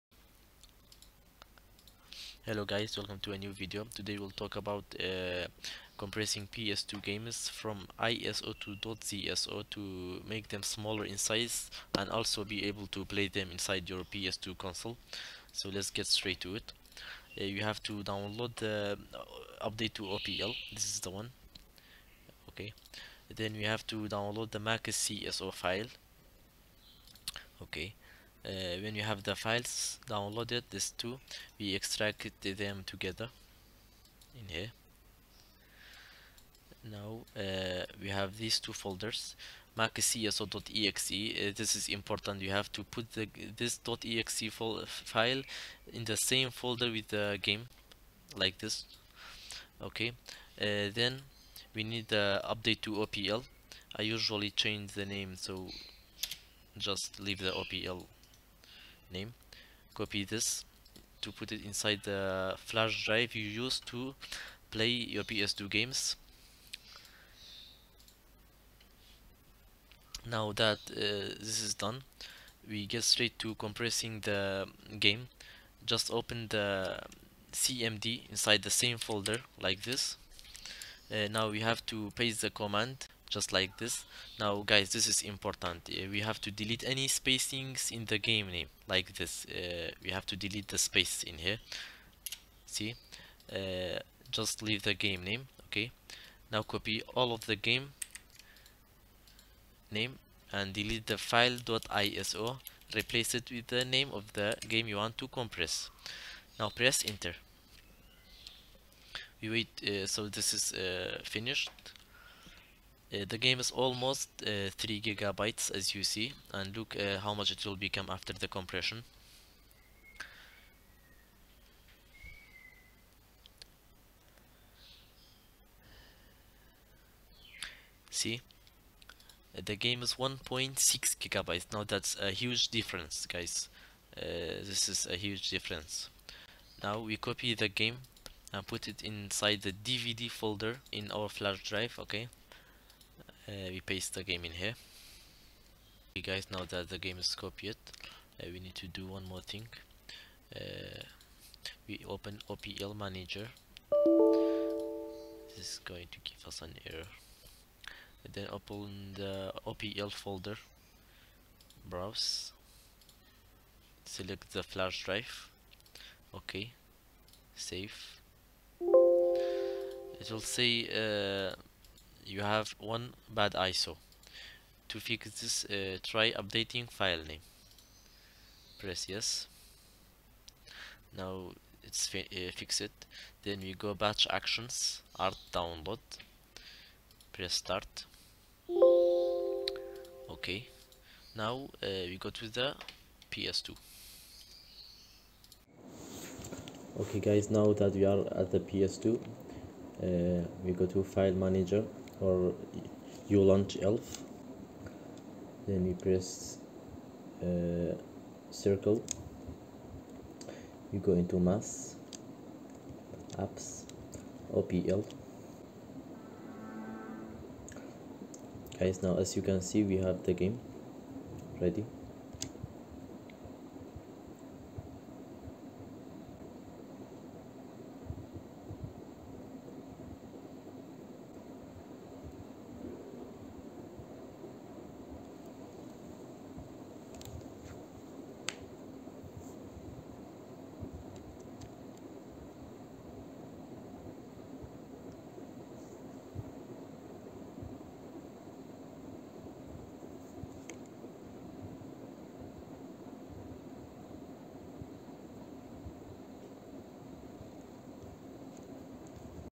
Make PS2 games smaller in size [ISO to ZSO] [OLD METHOD] [NEW METHOD IN DESCRIPTION]
NEW METHOD: Download opl manager program for windows and in the tools section there is an option to convert iso to ziso

maxcso --block=2048 --format=zso file.iso